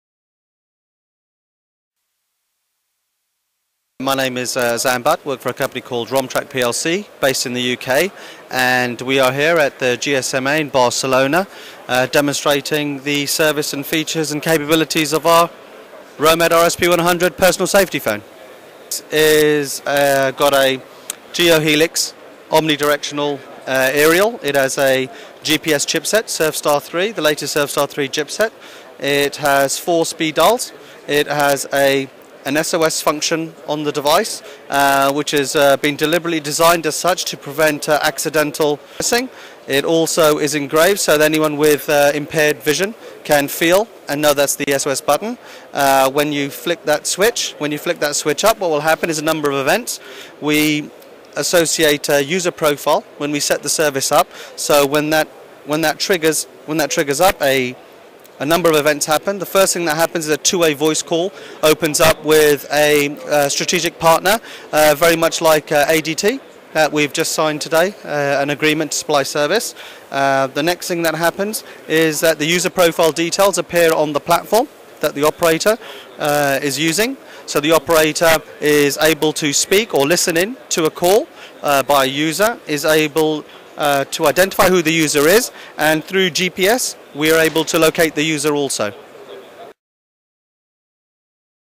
Romtrac to showcase personal safety phone 'Romad RSP-100'
Romtrac showcased its personal safety phone 'Romad RSP-100' at the MWC 2008 held in Barcelona, Spain.

The Romad RSP-100 comes with 4 pre-programmable speed dial numbers and a rugged SOS alert button providing powerful alert and response capability through communication, location, identification and response through accredited 24/7 Alarm Receiving Centres. It is suitable for deployment in any lone worker, security or workforce management environment while weighing only 62.50g with the standard battery attached.